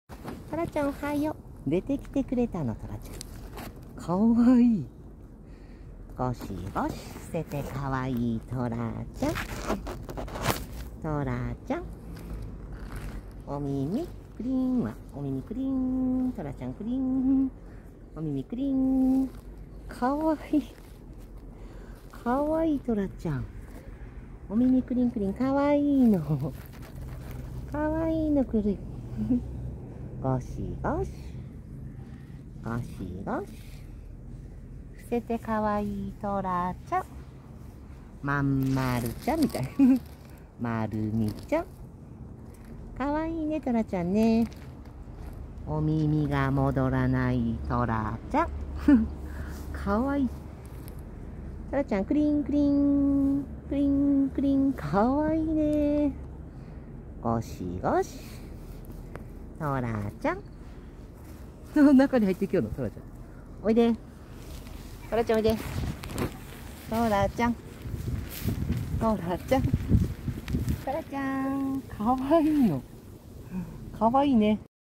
出て来てくれたお耳クリンクリンのとらちゃん❗伏せた姿と丸っこいところが可愛いんだニャン❤️
出て来てくれたお耳クリンクリンのとらちゃん❗
伏せた姿と丸っこいところが可愛いんだニャン❤️
ゴシゴシ⤴️

Tora-chan with ears crinkling who came out❗
 The face down and roundness are cute, nyan❤️
 Scrubbing ⤴️